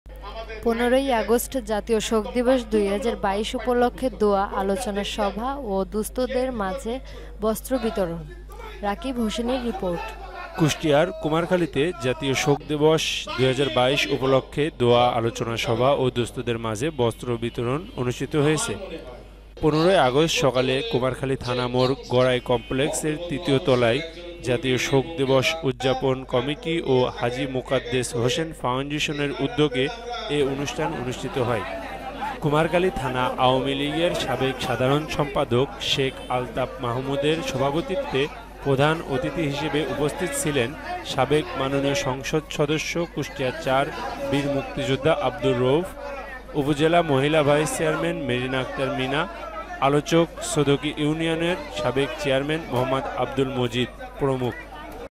১৫ই আগষ্ট জাতীয় শোক দিবস ২০২২ উপলক্ষে দোয়া, আলোচনা সভা ও দুস্থদের মাঝে বস্ত্র বিতরণ
১৫ই আগষ্ট জাতীয় শোক দিবস ২০২২ উপলক্ষে দোয়া, আলোচনা সভা ও দুস্থদের মাঝে বস্ত্র বিতরণ

কুষ্টিয়ার কুমারখালীতে জাতীয় শোক দিবস ২০২২ উপলক্ষে দোয়া, আলোচনা সভা ও দুস্থদের মাঝে বস্ত্র বিতরণ অনুষ্ঠিত হয়েছে।
১৫ই আগষ্ট সকালে কুমারখালী থানা মোড় গড়াই কমপ্লেক্সের ৩য় তলায় জাতীয় শোক দিবস উদযাপন কমিটি ও হাজী মোকাদ্দেস হোসেন ফাউন্ডেশনের উদ্যোগে এ অনুষ্ঠান অনুষ্ঠিত হয়।
কুমারখালী থানা আওয়ামী লীগের সাবেক সাধারণ সম্পাদক শেখ আলতাফ মাহমুদের সভাপতিত্বে প্রধান অতিথি হিসেবে উপস্থিত ছিলেন সাবেক মাননীয় সংসদ সদস্য কুষ্টিয়া-৪ বীর মুক্তিযোদ্ধা আব্দুর রউফ, উপজেলা মহিলা ভাইস চেয়ারম্যান মেরিনা আক্তার মিনা, আলোচক সদকী ইউনিয়নের সাবেক চেয়ারম্যান মো আব্দুল মজিদ প্রমূখ।